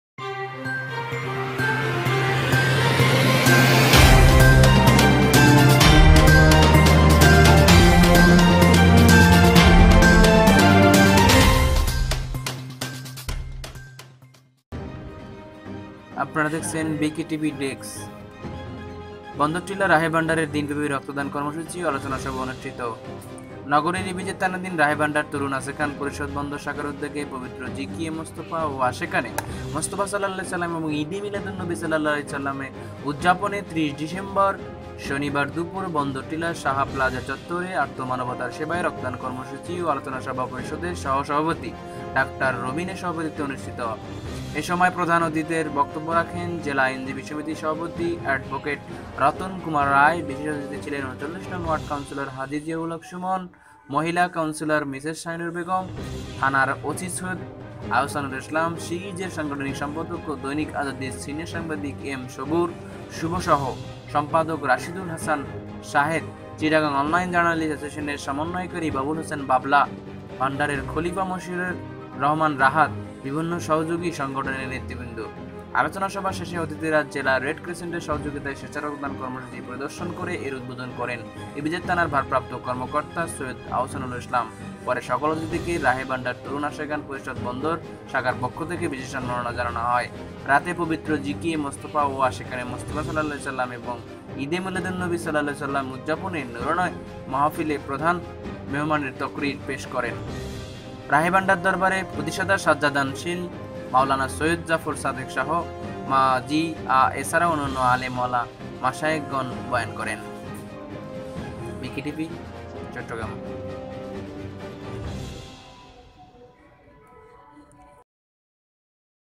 ইপিজেড থানাধীন রায়ে ভাণ্ডার আশেকান পরিষদের উদ্দ্যোগে স্বেছায় রক্তদান কর্মসুচী ও আলোচনা সভা। bk tv
BK Tv বিশ্বব্যাপী অনলাইন টেলিভিশন।